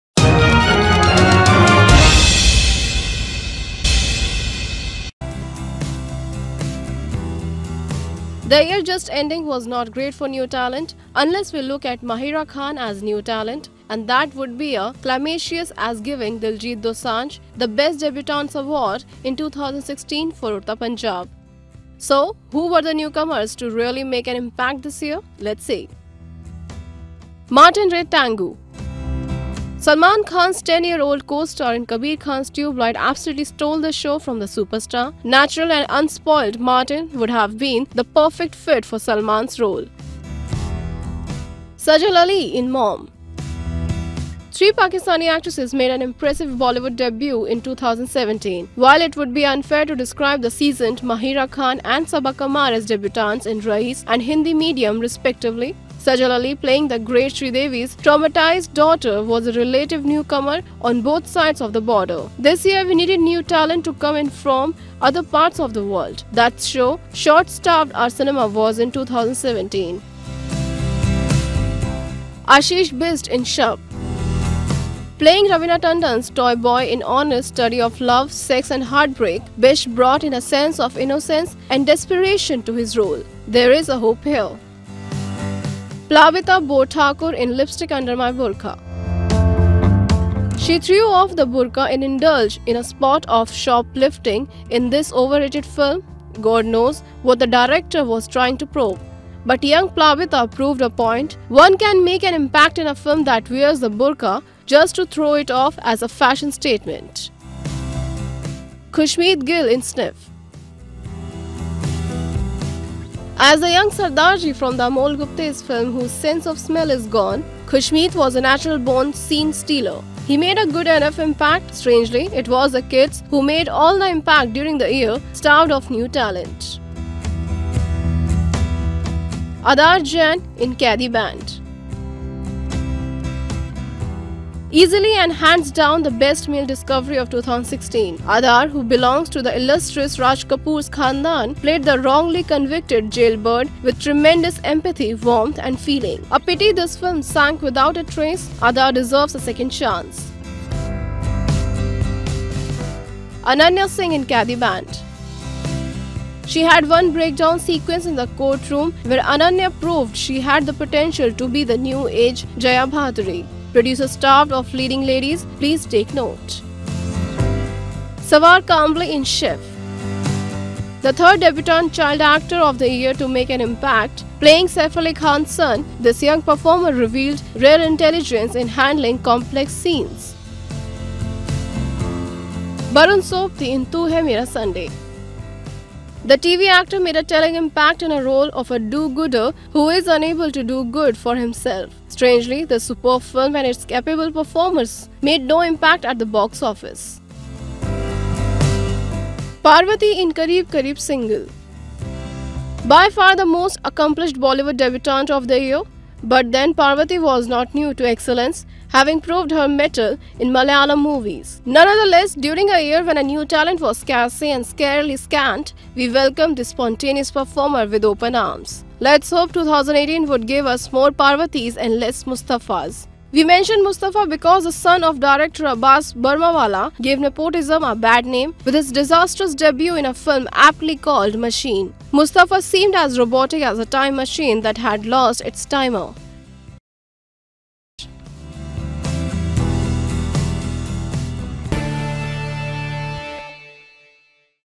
In Retrospect of 2017: Newcomers who made an impact in Bollywood
The year just-ending was not great for new talent. Unless we look at Mahira Khan as new talent. And that would be as calamitous as giving Diljit Dosanjh the best debutants award in 2016 for "Udta Punjab". Let's hope 2018 would give us more Parvathis and less Mustafas. We mention Mustafa because the son of director Abbas Burmwalla gave nepotism a bad name with his disastrous debut in a film aptly called "Machine". Mustafa seemed as robotic as a time machine that had lost its timer